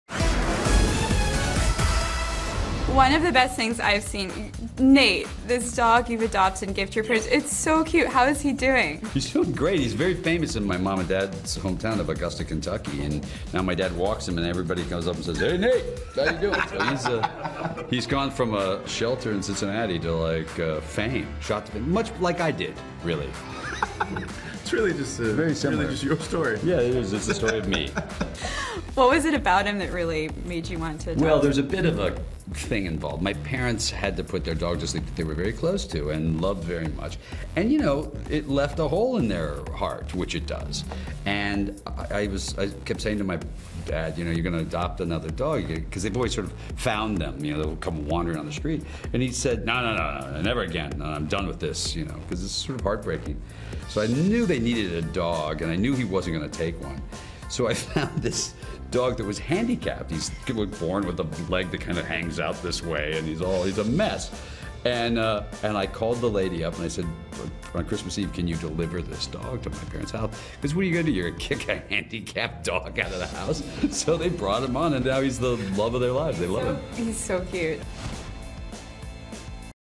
EXCLUSIVE: George Clooney Reveals Why He Adopted That Handicapped Dog For His Parents
A mutt named Nate is now a rockstar in Clooney's hometown!